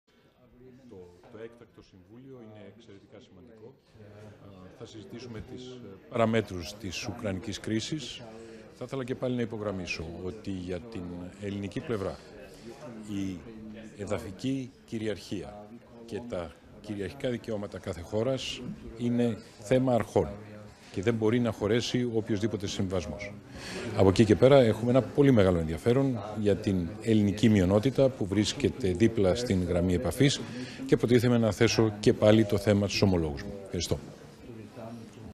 Δήλωση ΥΠΕΞ, Ν. Δένδια, κατά την άφιξή του στο έκτακτο άτυπο Σ.Ε.Υ. της ΕΕ (Παρίσι, 22.02.2022)
Δήλωση Υπουργού Εξωτερικών, Νίκου Δένδια, κατά την άφιξή του στο έκτακτο άτυπο Συμβούλιο Εξωτερικών Υποθέσεων της ΕΕ (Παρίσι, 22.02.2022)